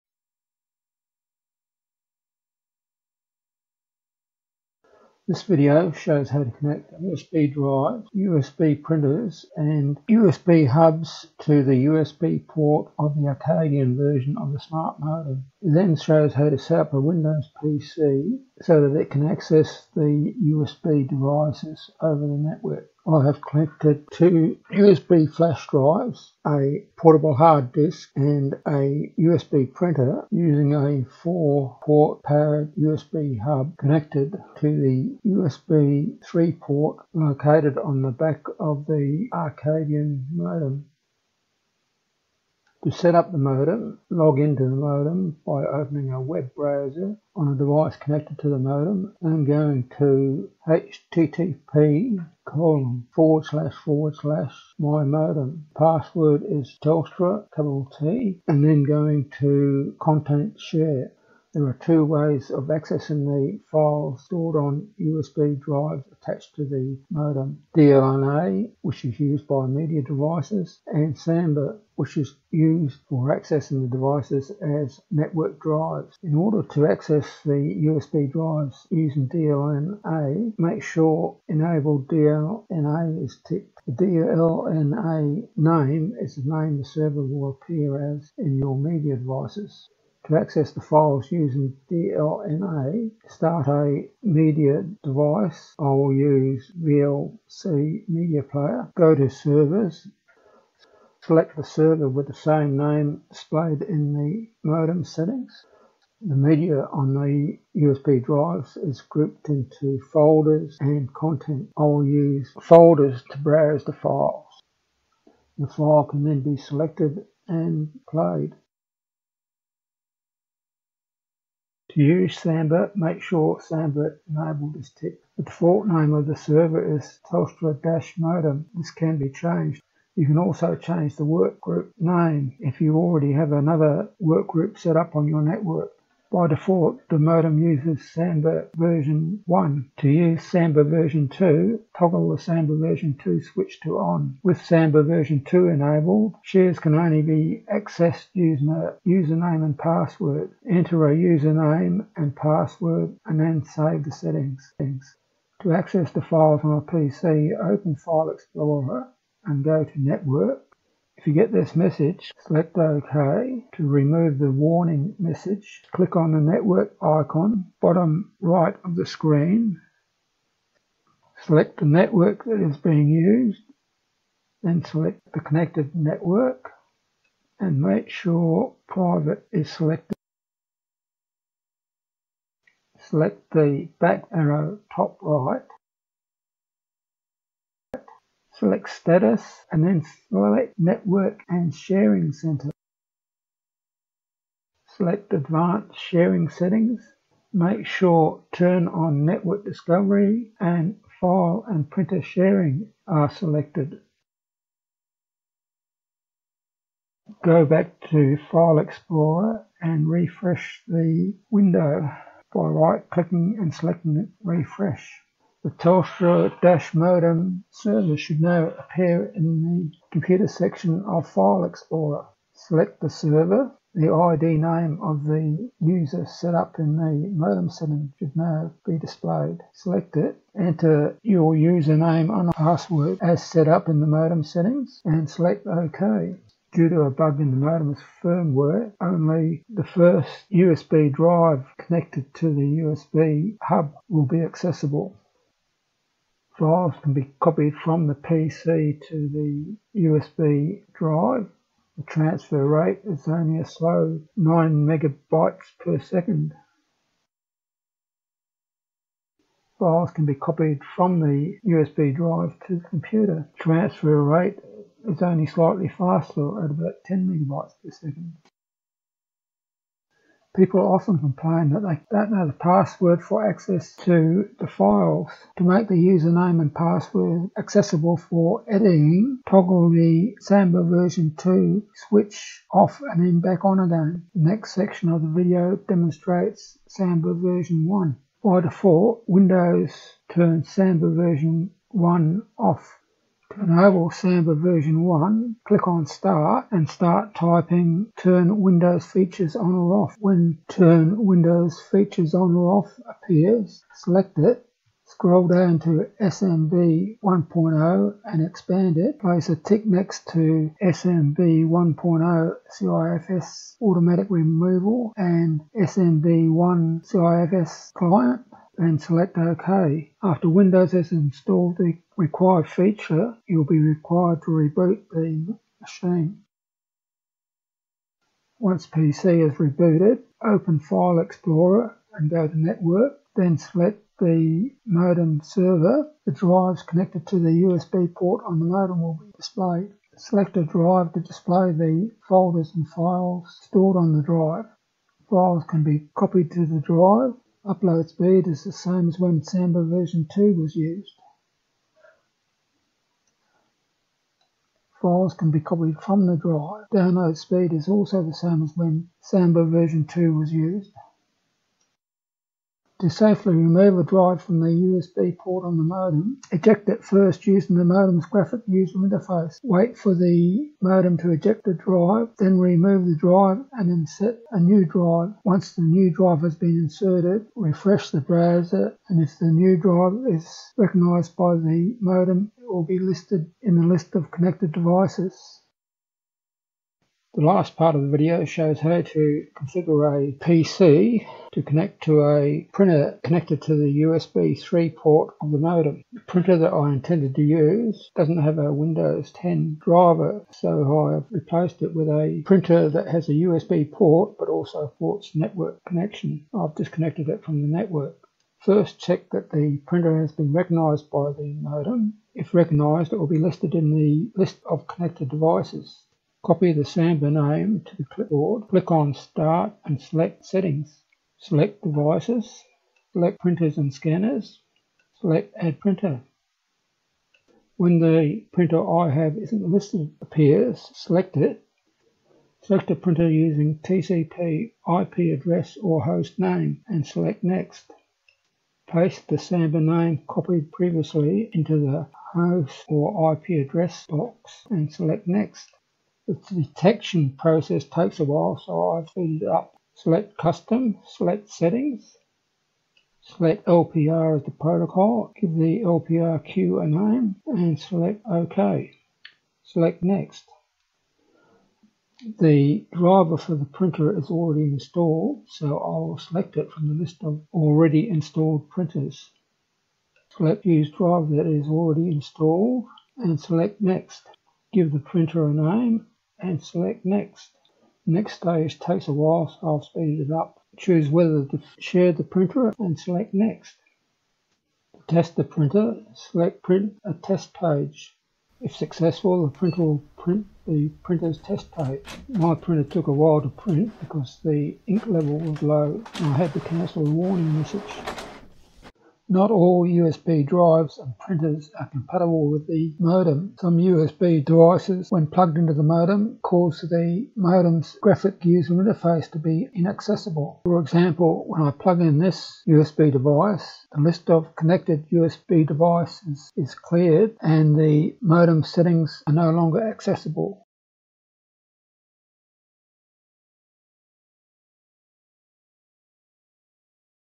Connecting USB Devices to the Arcadyan Version of the Telstra Smart Modem Gen 2
This video shows what devices can be connected to the USB port of the Arcadyan version of the Telstra Smart Modem, how to set up the modem and how to set up PC.

Video conntents:
00:00  Intro
00:44  Setting up Modem
01:20  Media Server (DLNA)
02:00  File Server (SMB2) setting up modem
02:37  Accessing Modem's File Server (SMB2) from PC
04:50: Accessing Modem's File Server (SMB1) from PC
06:30  Accessing USB printer connected to USB port from a PC.
08:18 Effects of incompatible USB devices on modem's GUI.